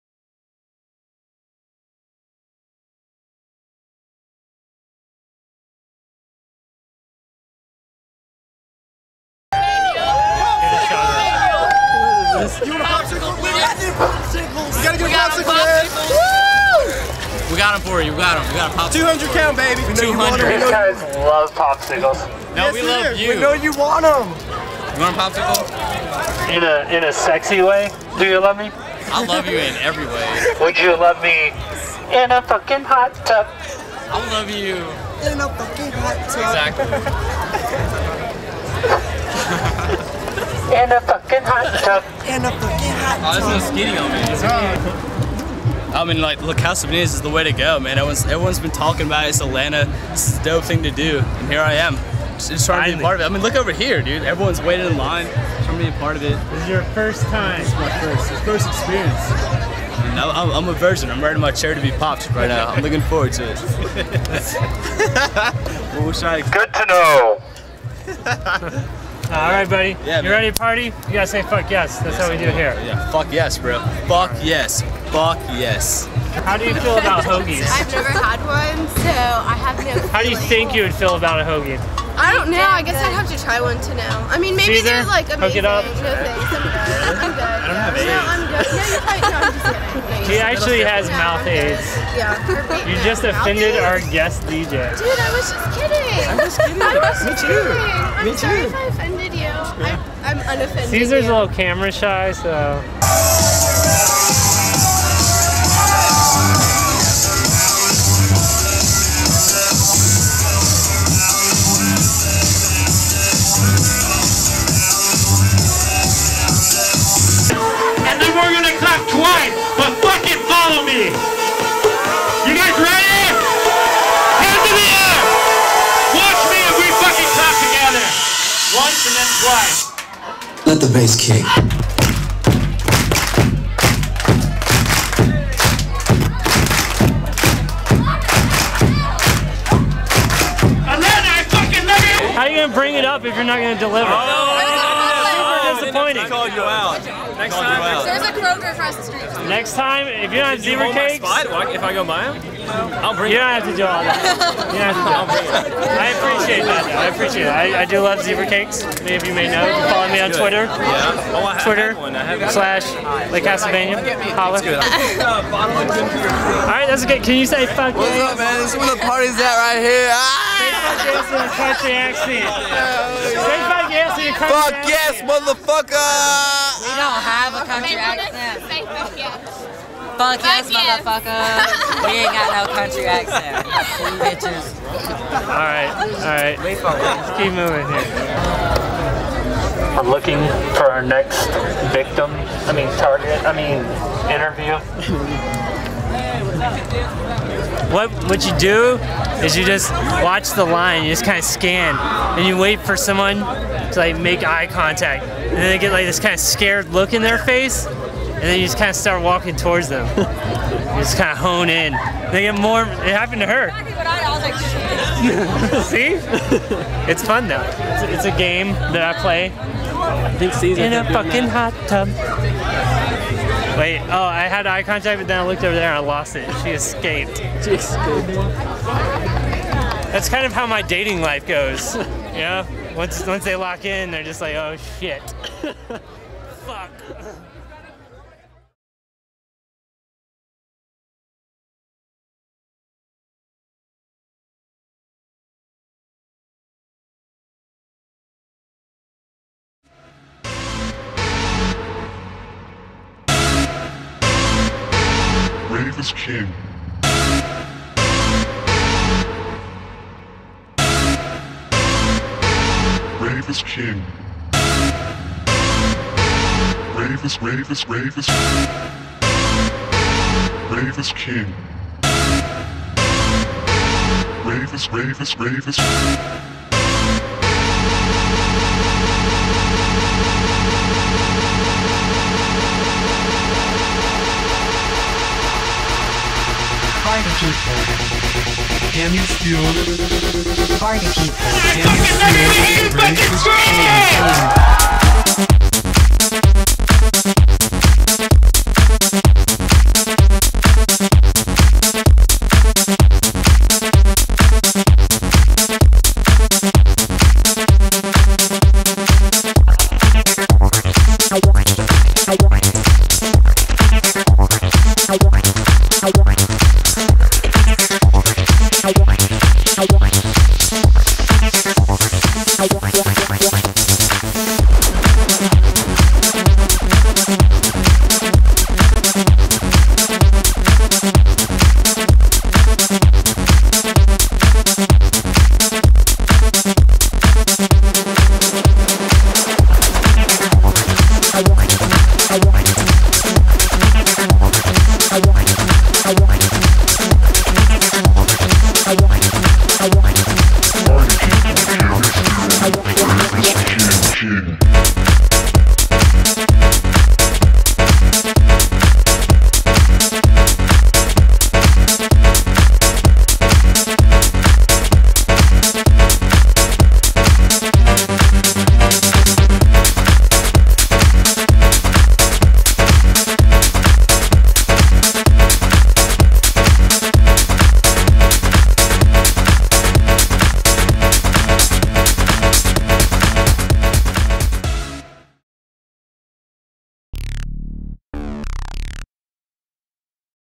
FUCK YESSS October 09 with LE CASTLE VANIA & PAPARAZZI
FUCK YESSS DANCE PARTY in Atlanta every first thursday of the month at the Drunken Unicorn!